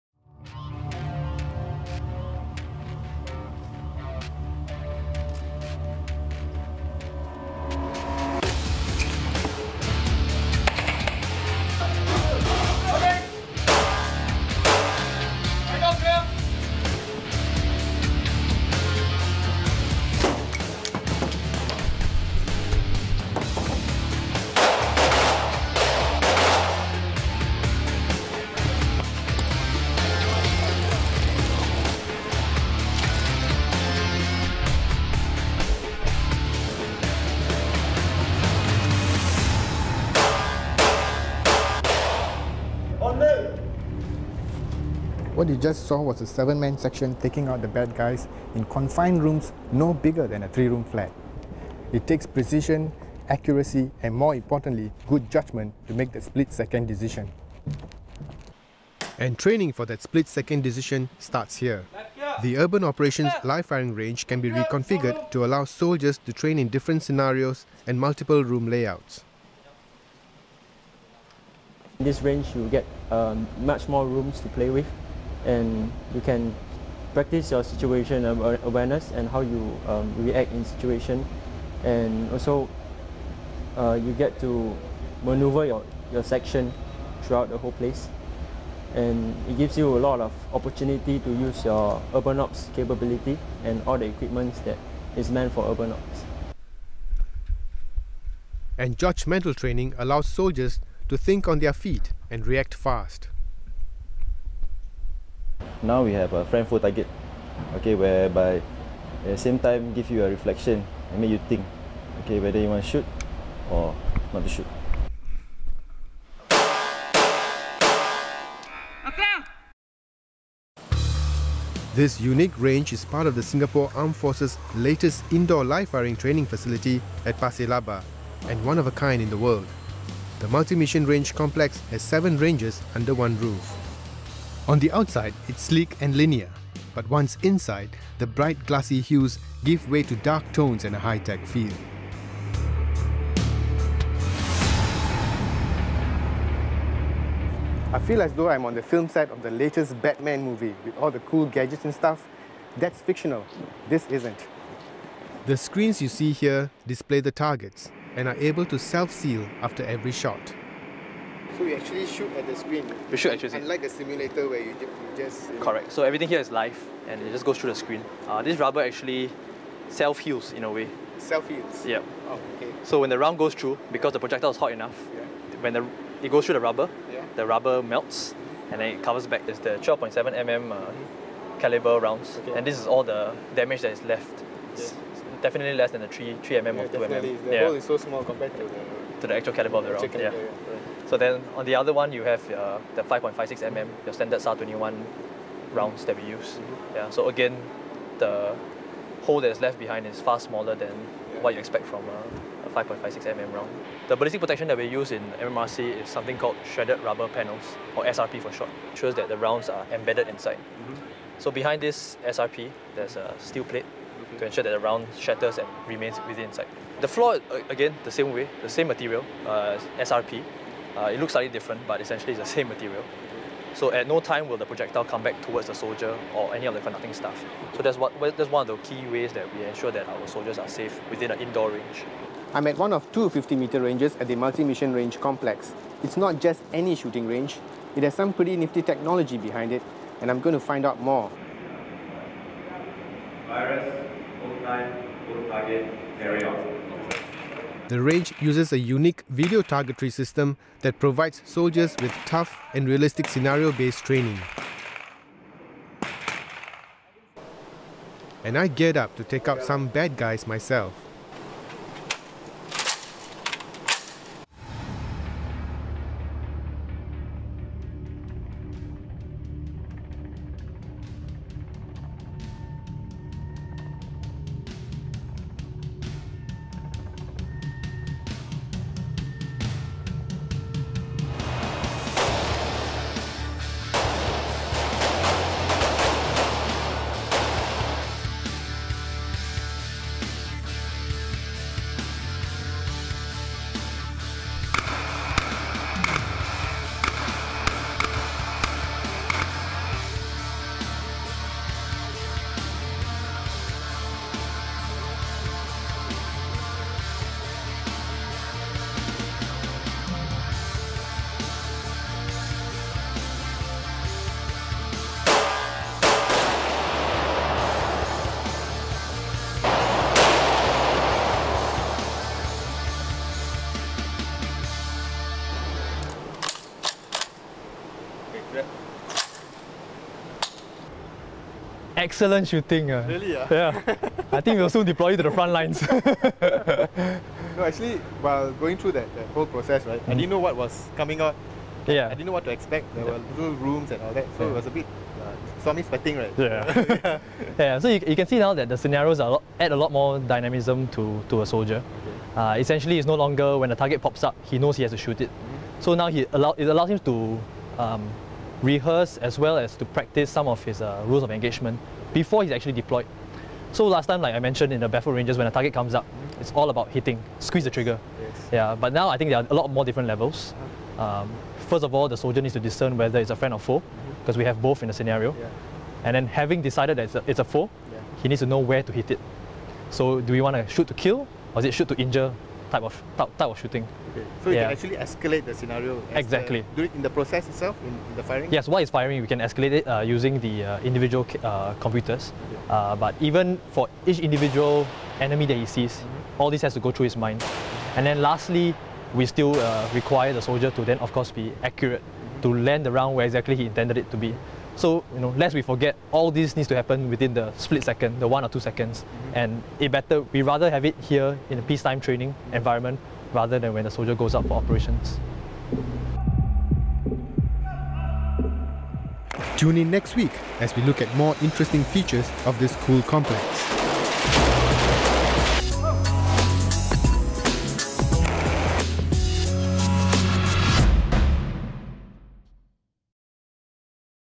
Ep 1: Right on Target! (Multi-Mission Range Complex)
The SAF's latest high-tech training facility not only sharpens our soldiers' shooting skills, it also develops their ability to think on their feet and react fast, picking out friend from foe.

And to make each round, count.

Filmed by: Peter Cho, James Teh

Edited and Produced by: Mano Mahendran

Audio post-production by: Kevin Tan

Graphics and Color Grading by: Lam Meng Chuen